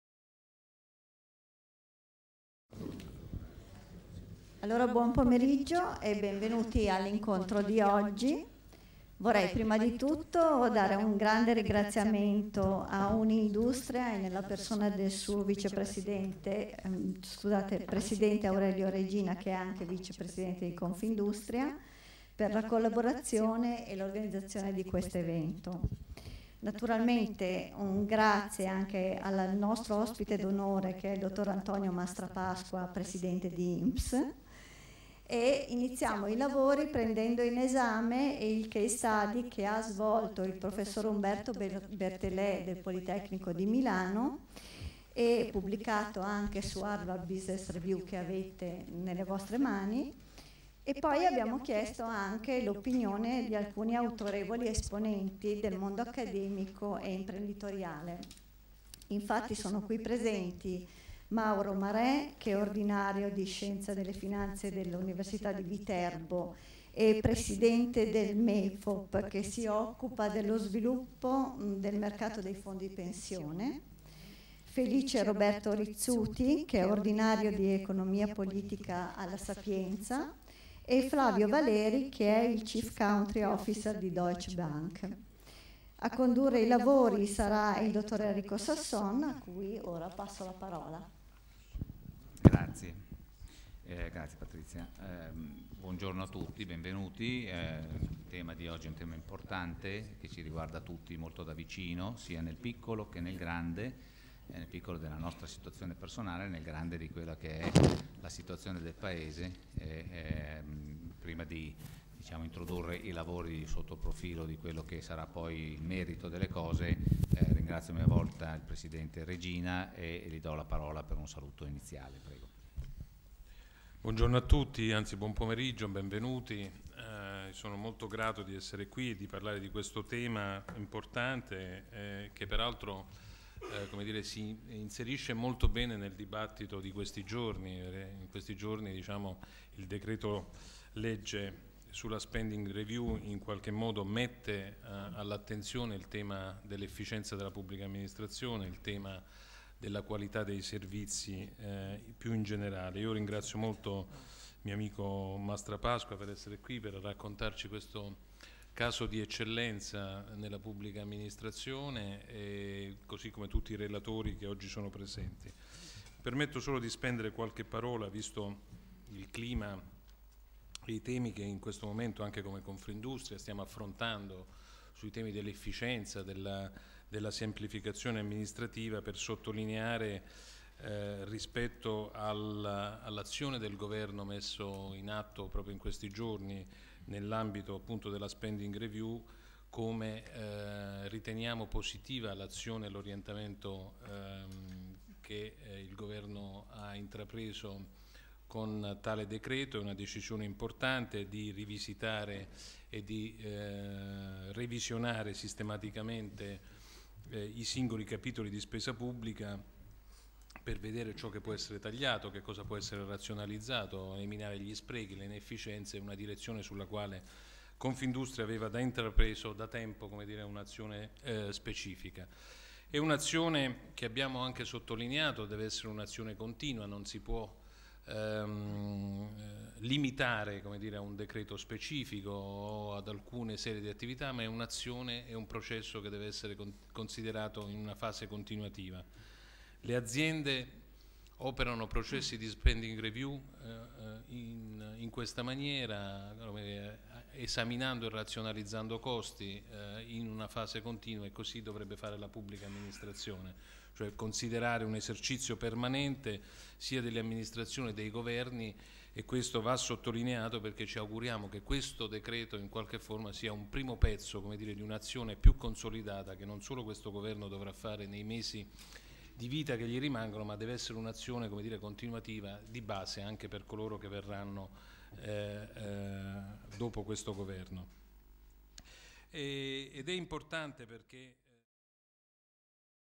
La modernizzazione della Pubblica Amministrazione per la competitività del Paese: il caso INPS
Nell'attuale quadro congiunturale uno dei fattori prioritari per favorire la competitività delle imprese e accelerare il risanamento economico del Sistema Paese è rappresentato dalla modernizzazione della Pubblica Amministrazione.
Unindustria - in collaborazione con "The Ruling Companies Association" e "Harvard Business Review Italia" - promuove l'incontro sul tema "La modernizzazione della pubblica amministrazione per la competitività del paese: il caso INPS".
L'iniziativa, che ha registrato gli interventi tra gli altri del Vice Presidente Confindustria con delega allo Sviluppo Economico Aurelio Regina e del Presidente INPS Antonio Mastrapasqua, intendeva approfondire la particolare materia partendo dal presupposto che per realizzare gli obiettivi di modernizzazione, nella previdenza, nella sanità, nel sistema fiscale e in quello della giustizia è necessario avere chiarezza di obiettivi, volontà di trasformazione e di attuare  un'azione efficace in tempi ragionevoli.
In occasione dell'incontro è stato presentato il Case Study "Un'INPS più efficiente per un'Italia più competitiva", pubblicato su Harvard Business Review Italia nel giugno 2012.